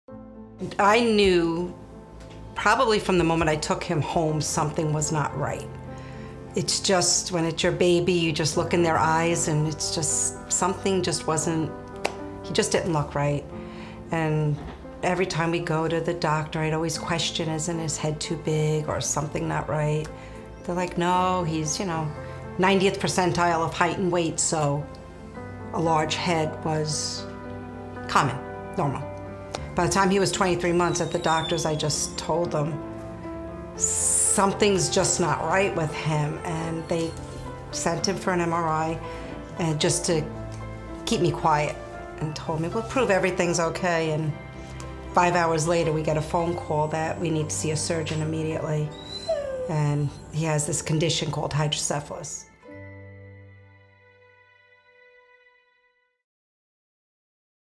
Finding Out Your Loved One Has Hydrocephalus
A mother never forgets. Mia Padron, a volunteer for the Hydrocephalus Association, recalls the moment she found out about her son's hydrocephalus diagnosis.